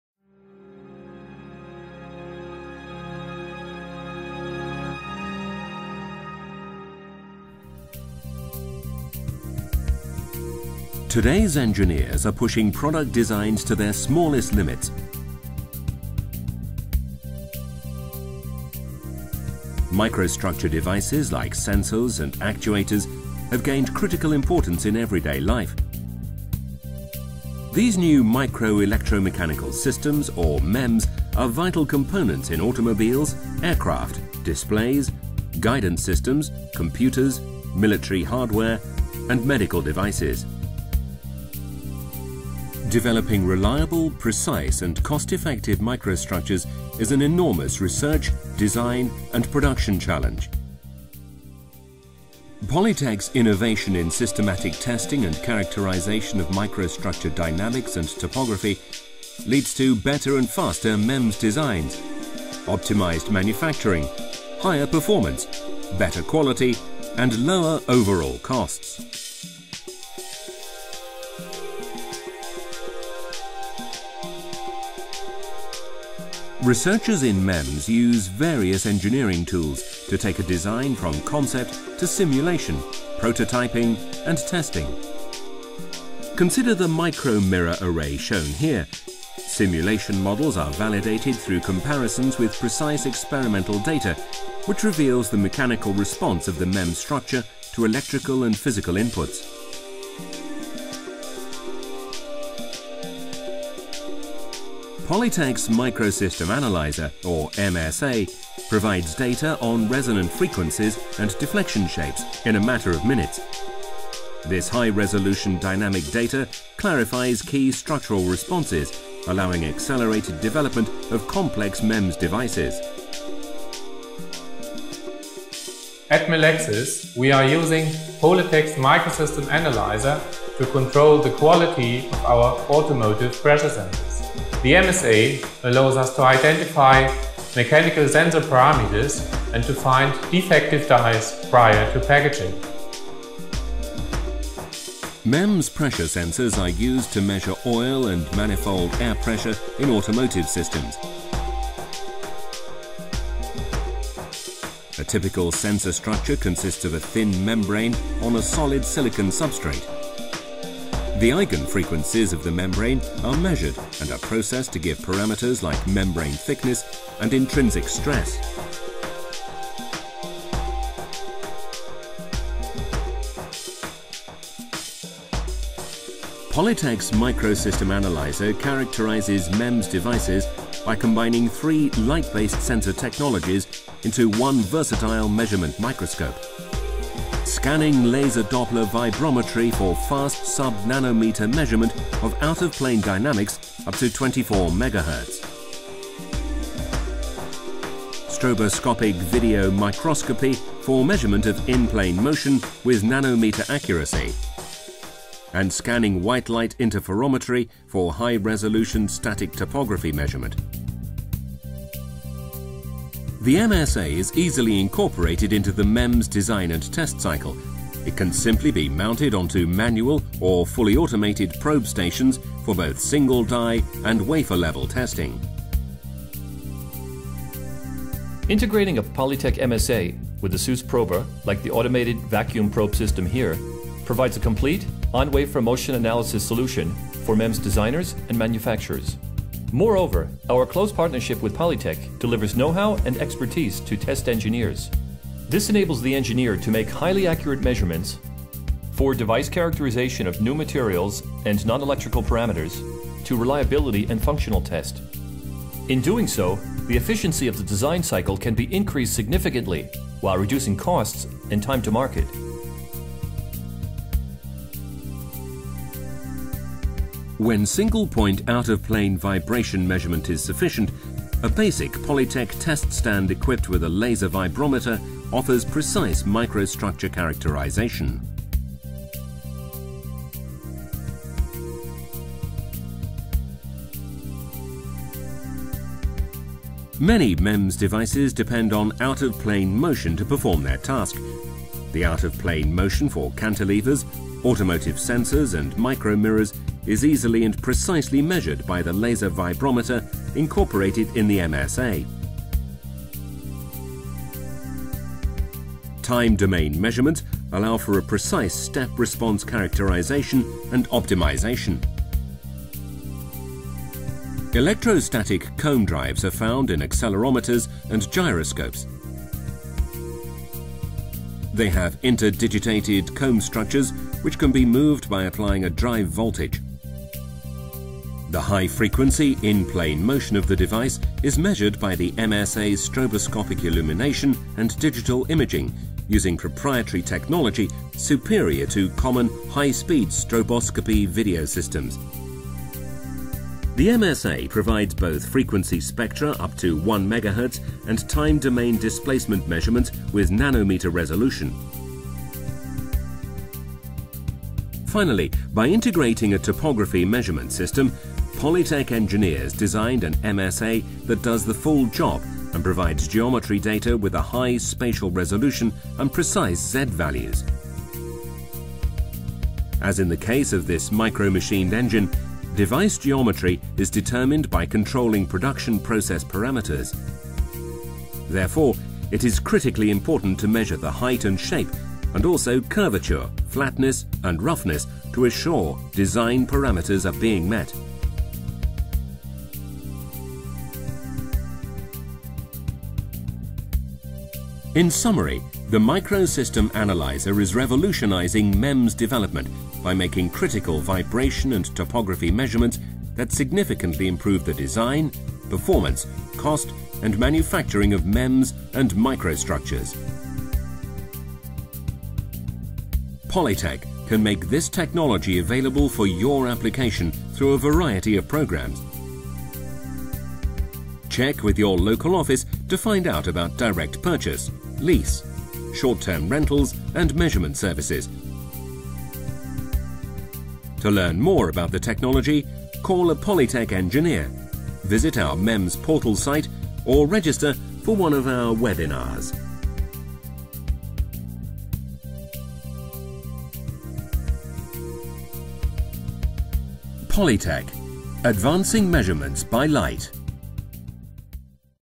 Microstructure Characterization
This video introduces the easy way to measure 3D dynamics and topography.